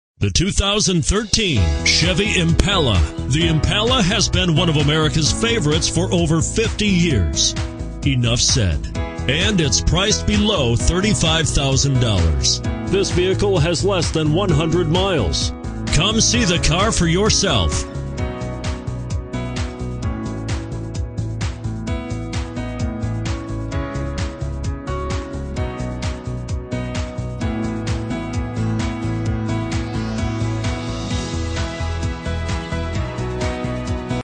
2013 CHEVROLET IMPALA White Bear Lake, MN 80212
2013 CHEVROLET IMPALA White Bear Lake, MN
Stock# 80212

1801 E Co Road F

Looking for the right car? Today could be your lucky day. With 8 miles, this Crystal Red Tintcoat, 2013 Chevrolet Impala equipped with Automatic transmission could be yours. Request more information and set up a test drive right away.